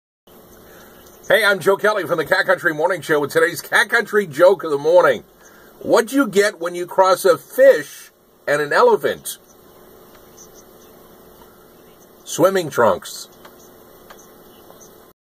What do you get? - Cat Country Joke of the Morning
A fish and an elephant? Find out what you get with this joke of the morning!